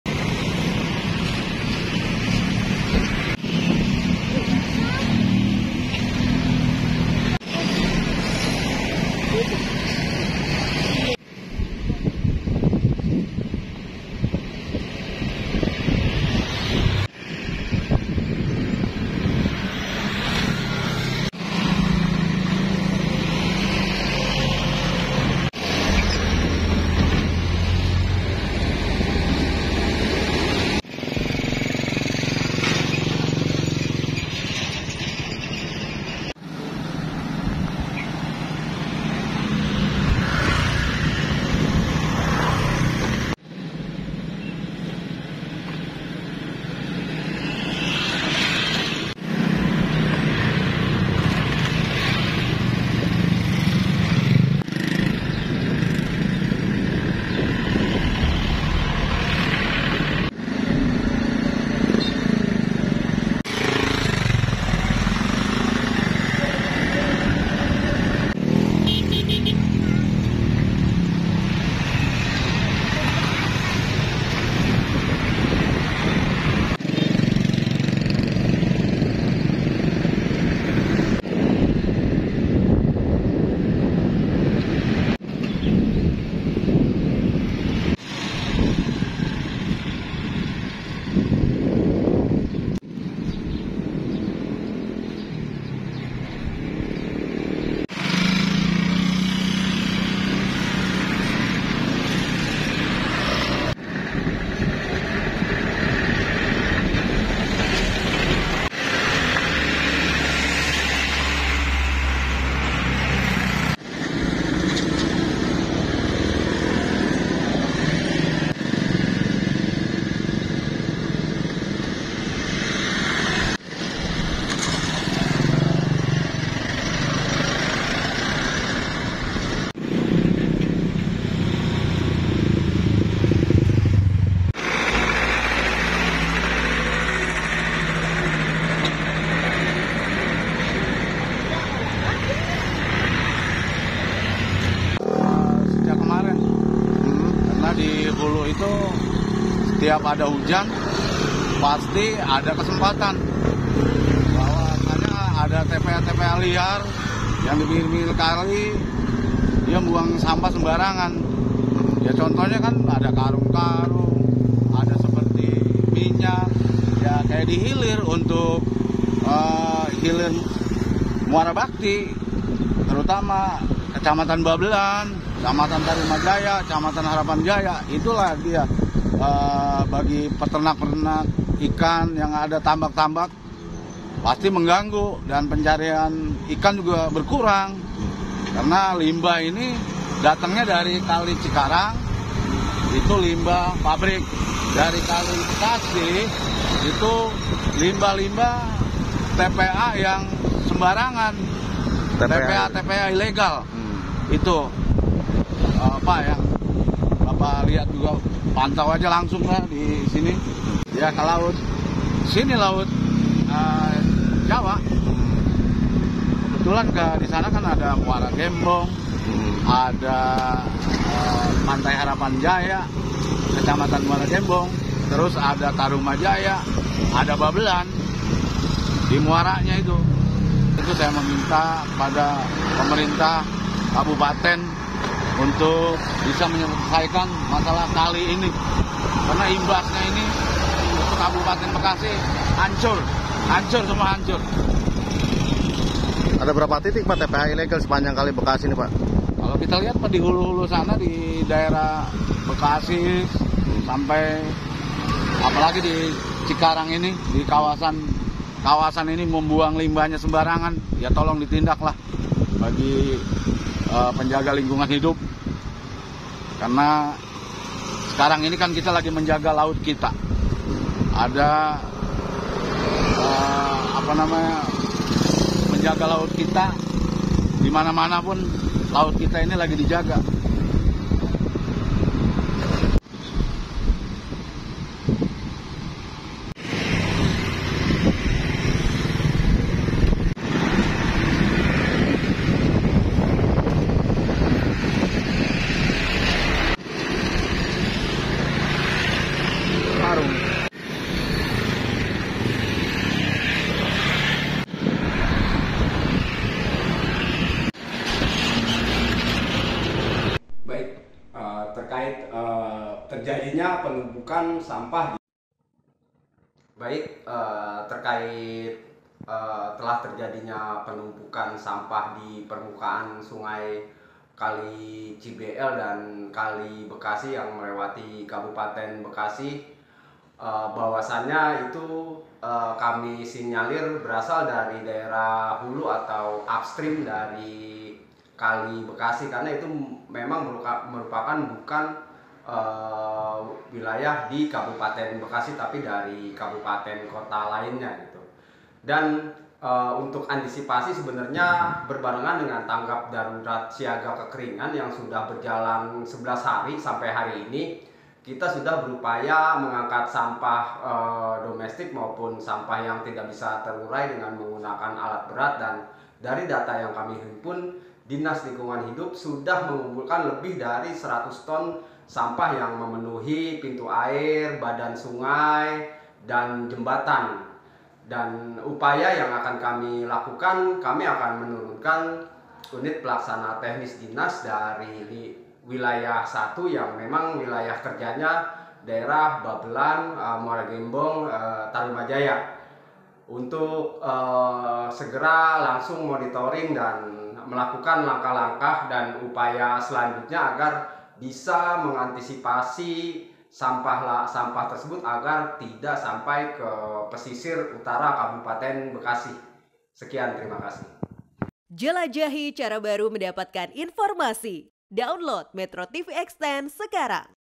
Viral! Sampah Membanjiri Permukaan Sungai Cikarang Bekasi Laut
MetroTV, Viral di media sosial ribuan kubik sampah membanjiri permukaan aliran sungai Cikarang Bekasi laut atau sungai CBL, selain tumpukan sampah, aliran sungai juga diduga tercemar limbah industri lantaran berwarga hitam pekat, sampah yang membanjiri aliran sungai akan terbuang ke laut dan mengalir ke tambak bilik warga.